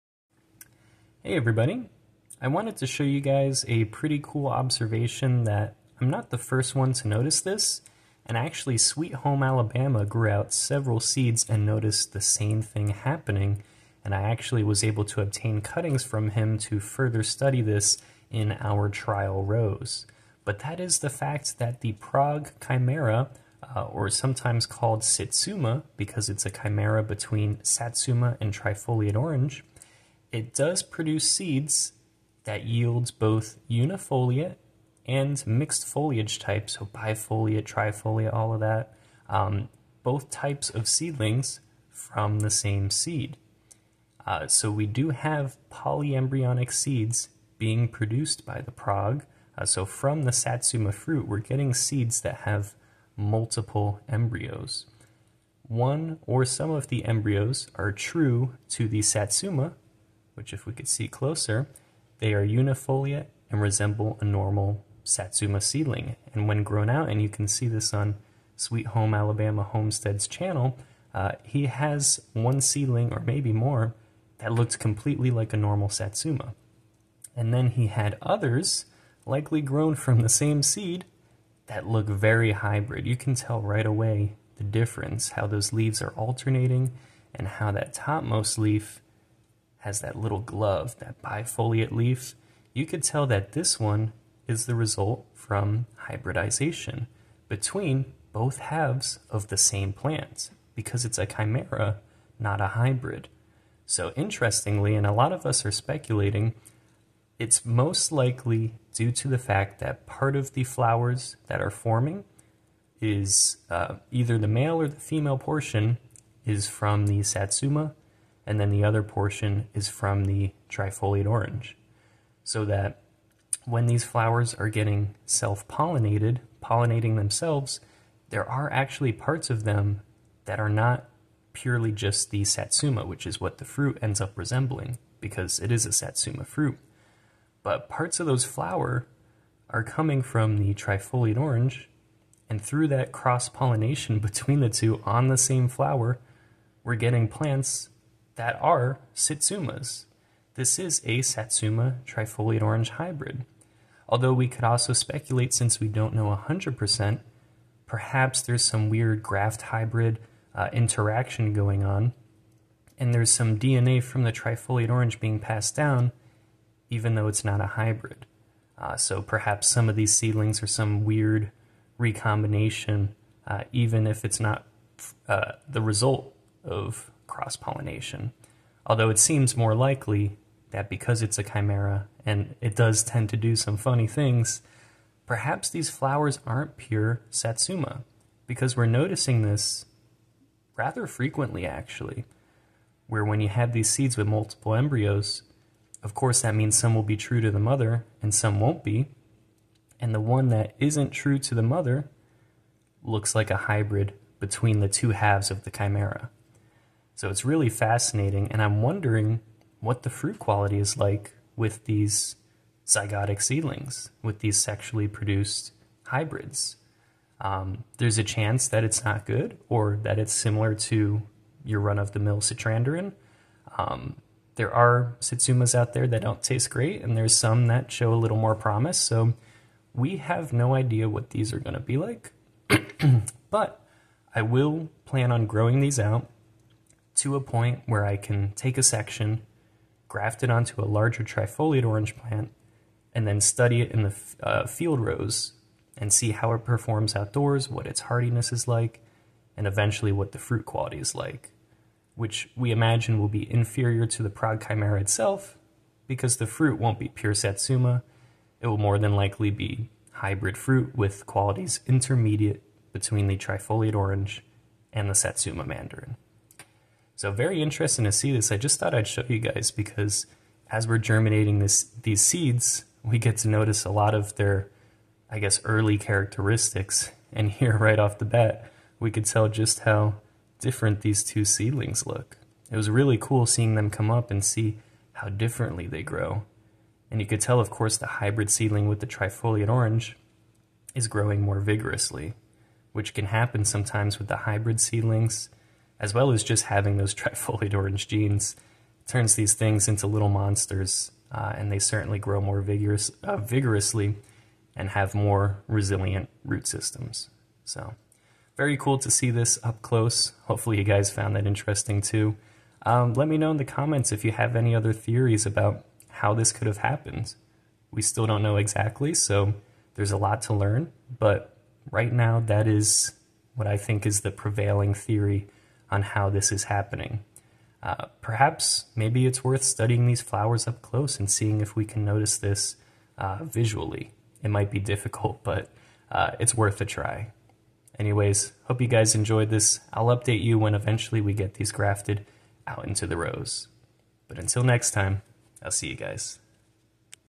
Prague Chimera Citrus Seedlings
Over the next few years, we will be cultivating these apparently hybrid "citsuma" seedlings that came from the seed of the Prague Chimera. Being a chimera, this technically shouldn't be possible unless the flowers also experience chimerism to some degree. Their hybrid nature seems more apparent since there are unifoliate and bifoliate/trifoliate seedlings emerging from the same seeds. Polyembryonic seeds can contain nucellar seedlings that are true-to-type, as well as zygotic seedlings that are sexually reproduced.